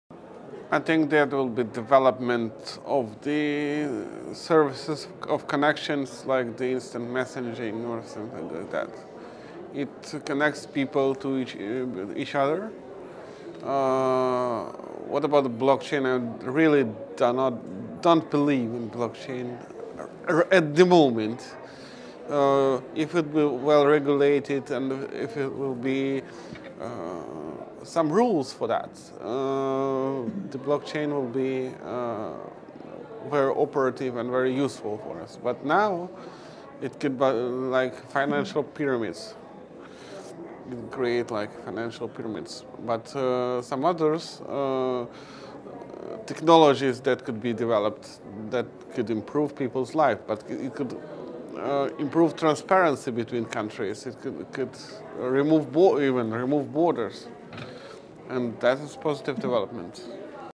Global IGF 2018: Andrey Shcherbovich on Best New Digital Development
Andrey Shcherbovich, lecturer, Higher School of Economics National Research University, Russia, shares their answer to the question, “What new digital technologies will create the best opportunities for a better world all over the next 10 years?” Recorded at the Global Internet Governance Forum 2018 in Paris, France, by the Imagining the Internet Center, an initiative of Elon University, North Carolina, USA.